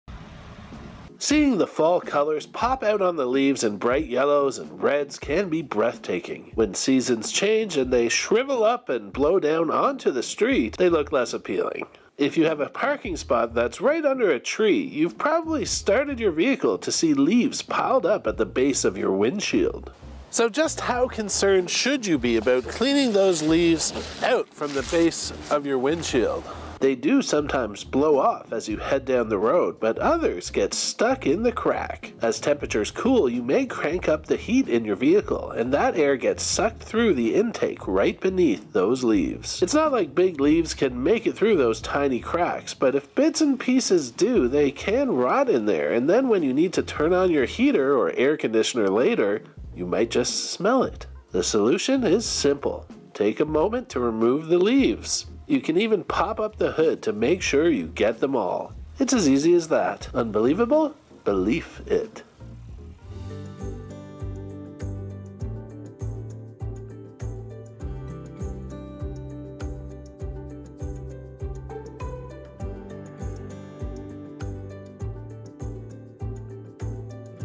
Why Those Fallen Leaves on Your Car Can Be a Nuisance if Left Alone
Nathan Coleman's TWN vehicle gathers some 'friends' overnight in the fall, but as he explains, leaving the leaves there isn't the best idea.